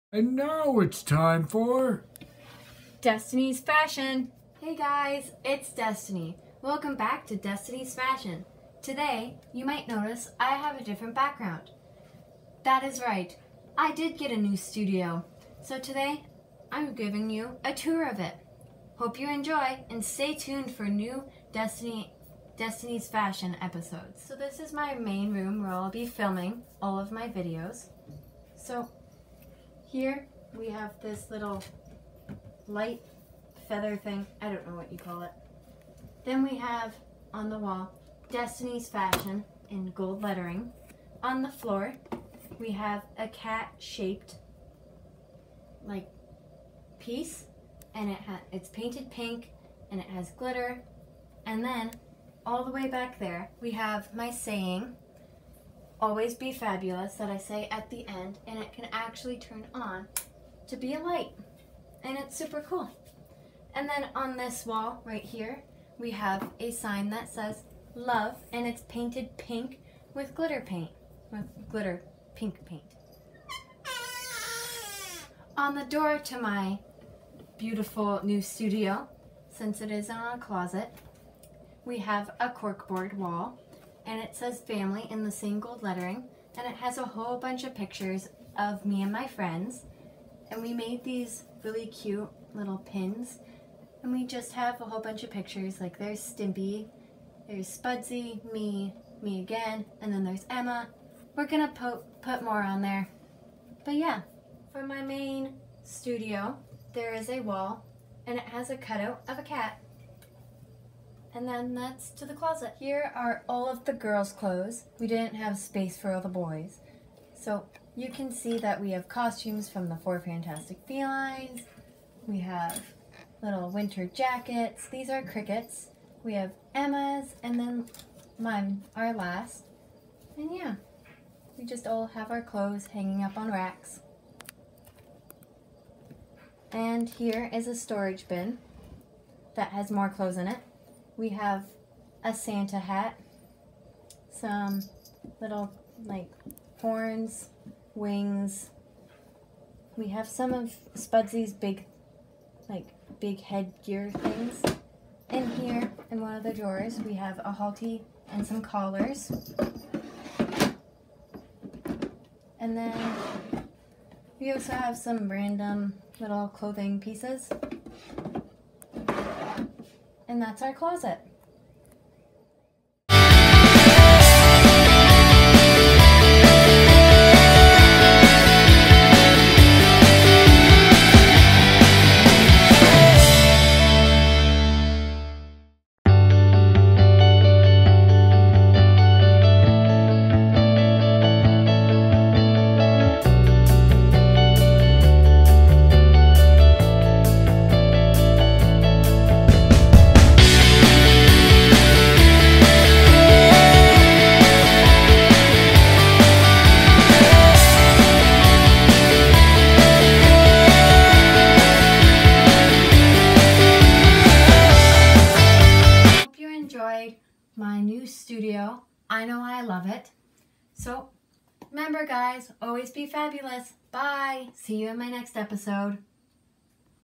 Destiney’s Fashion Episode 5- My New Studio
Hey everyone it’s Destiney I hope you enjoyed the tour of my new studio I love it so much thanks fur watching
~ Destiney